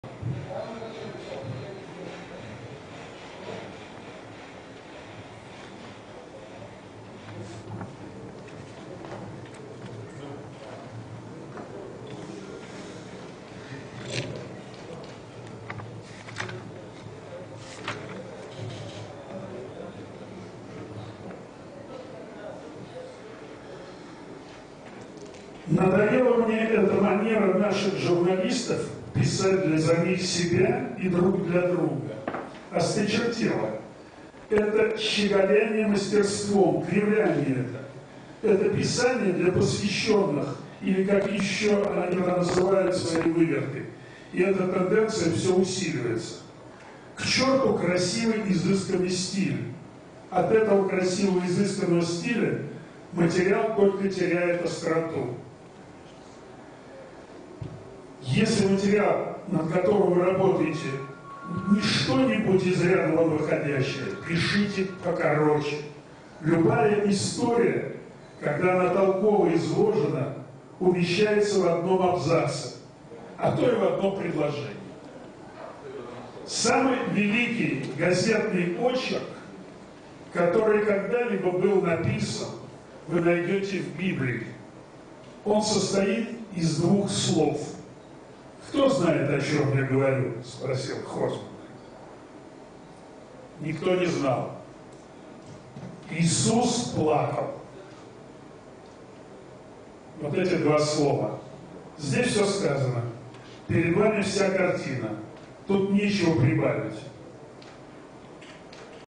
Seminar specializat pentru presa sportivă susținut de vicepreședintele AIPS-Europa, Lev Rossochik
Ascultă ce vorbesc oamenii deștepți...
Titlul evenimentului: Seminar specializat pentru presa sportivă susținut de vicepreședintele AIPS-Europa, Lev Rossochik